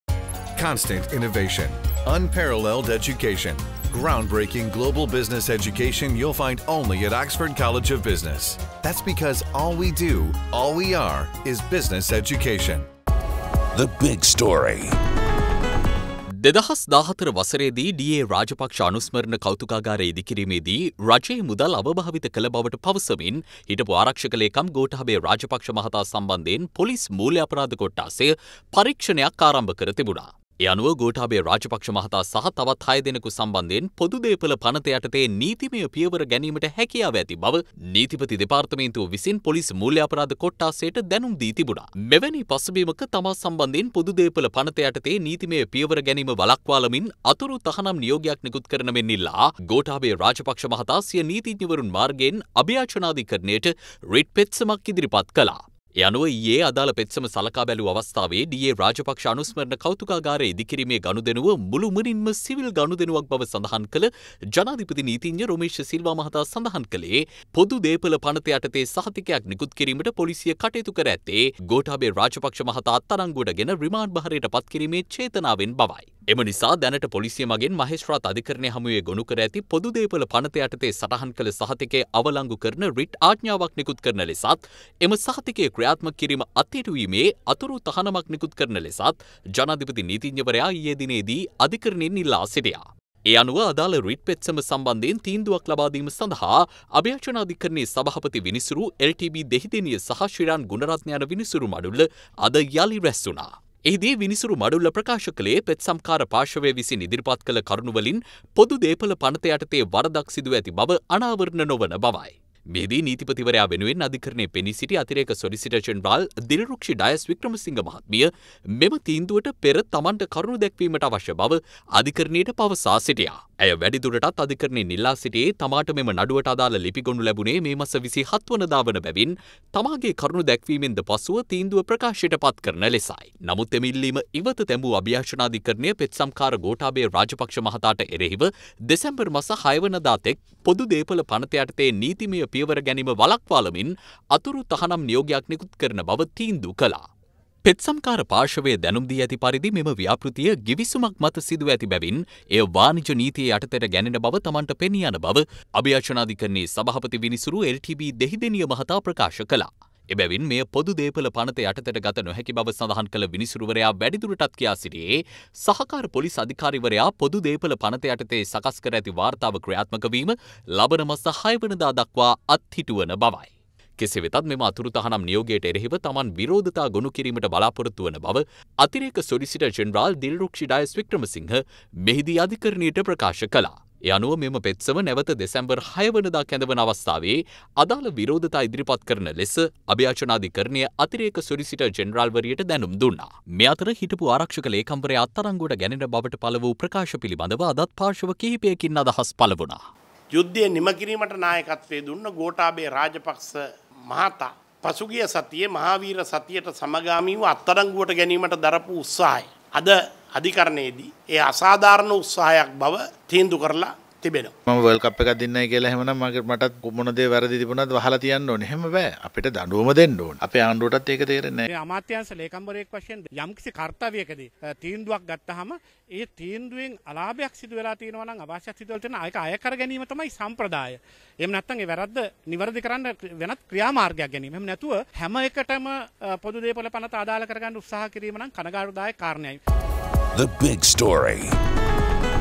Objections will be filed against interim order - ASG Dilrukshi Dias
අතුරු තහනම් නියෝගයට එරෙහිව විරෝධතා ගොනුකරනවා